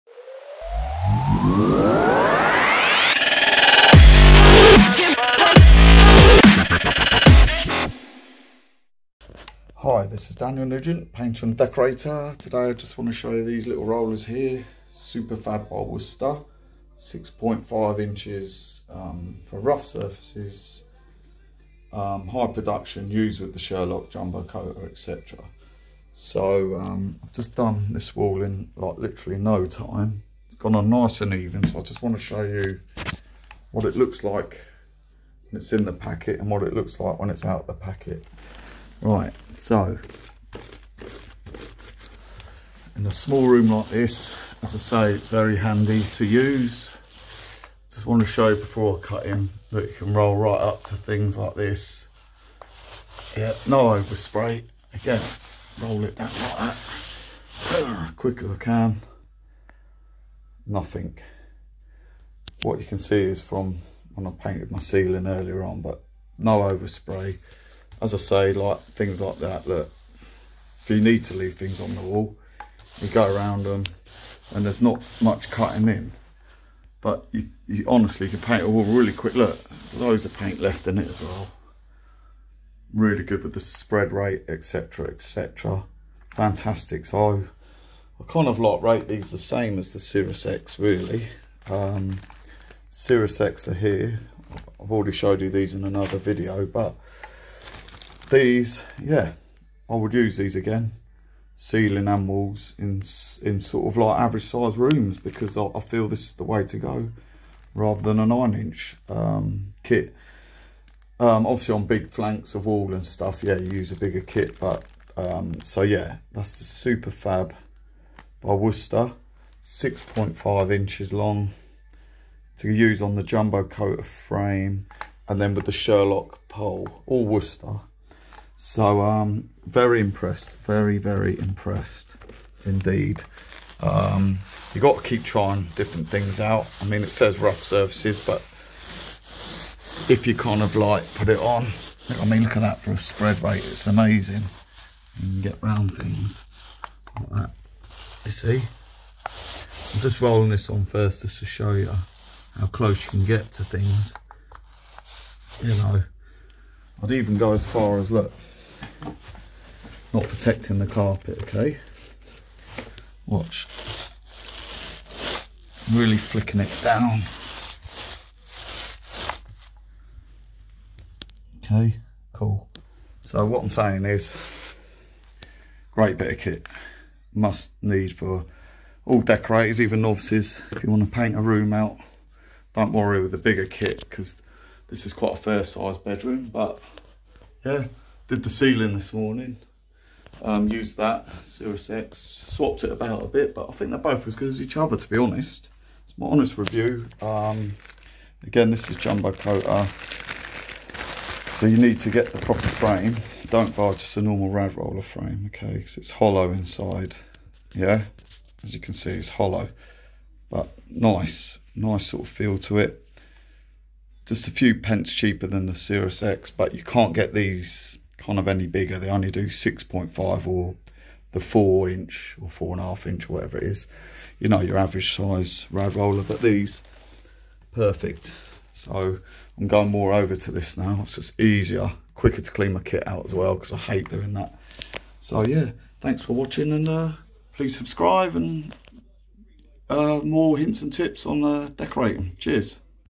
Super/Fab Roller By Wooster
This is a review on the Super/Fab roller by Wooster.
it's 6.5 inches in length and works with the small Wooster Jubo Koter frame and Sherlock pole also by Wooster.
I found it has amazing spred capacity and holds alot of paint without any over spray when rolling
You can roll so much closer to sockets etc so makes cutting in alot easier.
I would only use this in a matt finish as I think anything with a sheen your best off with a microfiber sleeve.
I did this room just as quick as I would with a bigger 9 inch kit.
It's less cumbersome and can be washed out after use alot quicker than bigger kit.